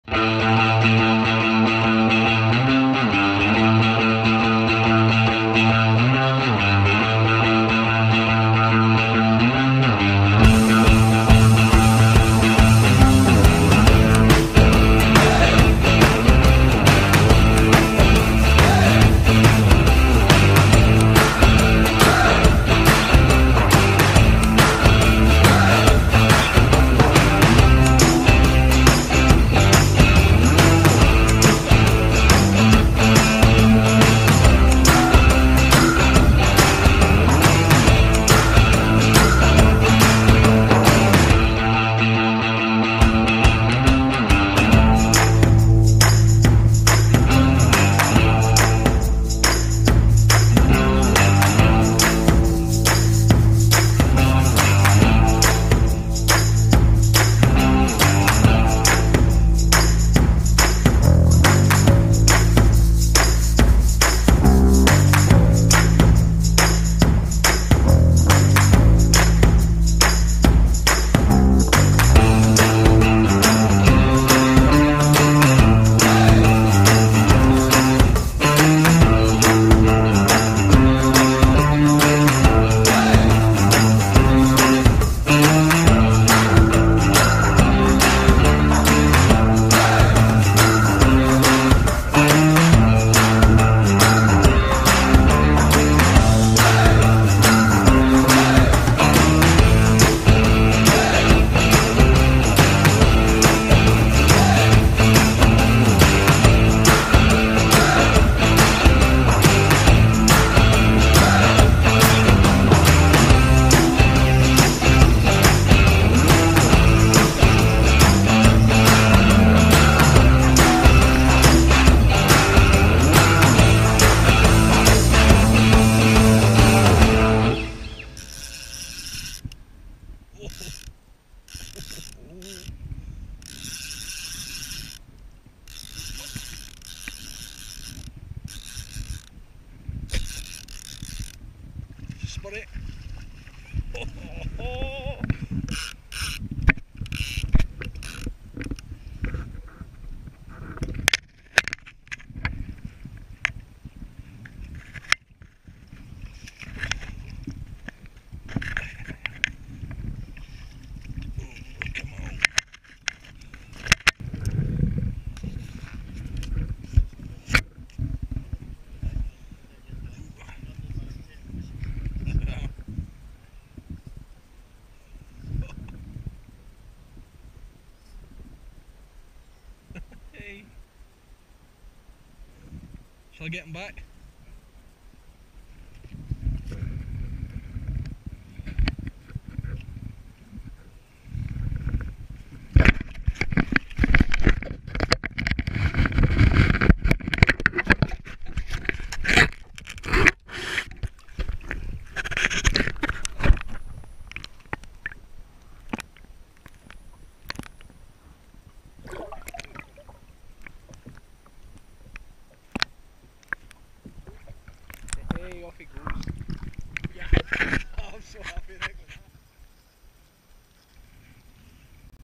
Spurdog encounters, Loch Etive Winter Kayak Fishing
short video of myself and Nicolas Valentin out fishing on Etive for the Spurdog